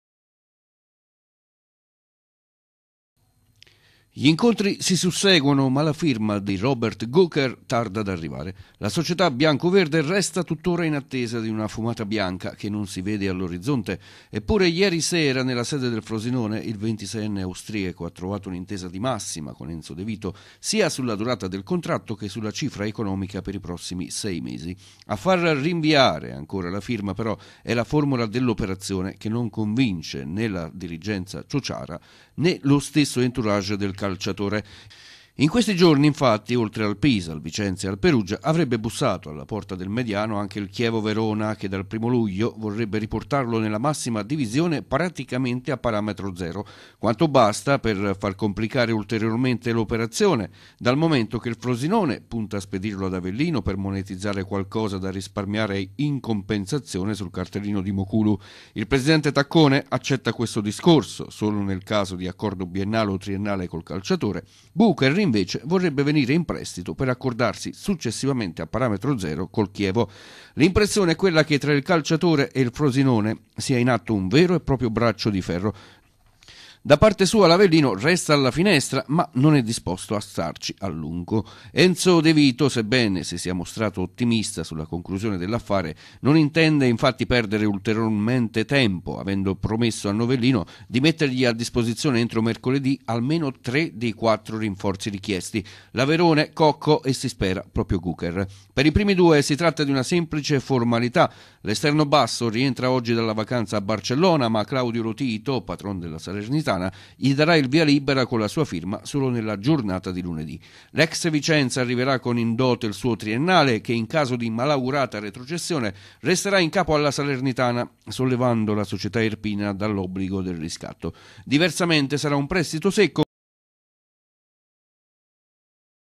GUCHER: COSI' VICINO, COSI' LONTANO
Positivo l'incontro tra la dirigenza biancoverde e l'entourage del calciatore ma nella trattativa si è inserito il Chievo Verona.